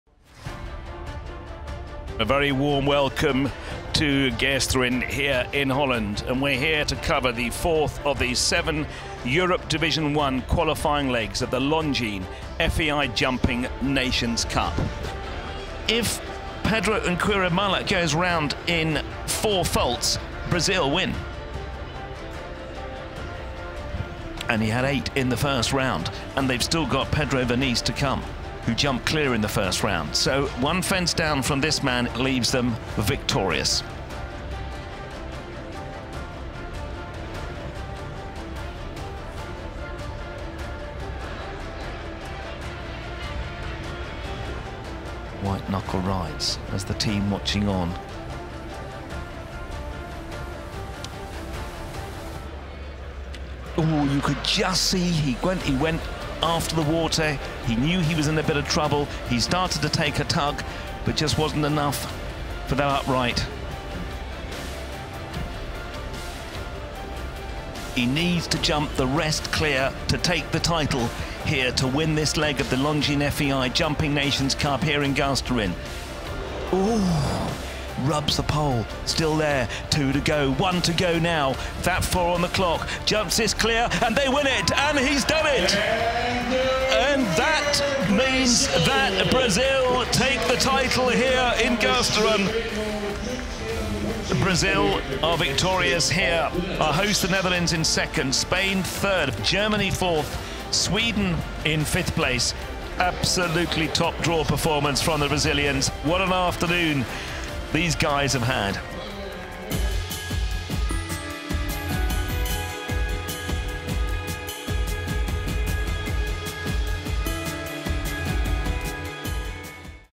Dutch team in 2nd on home soil - Brazil takes the win | Longines FEI Jumping Nations Cup™ 2019
It was a tough fight at the Longines FEI Jumping Nations Cup™ 2019 in Geesteren, and the home nation nearly made it to a victory on home soil. But it was the Team from Brazil, which perfectly manoeuvred every inch of the track in superior style to take maximum points.
3rd place in this international team jumping competition over two rounds went to Spain.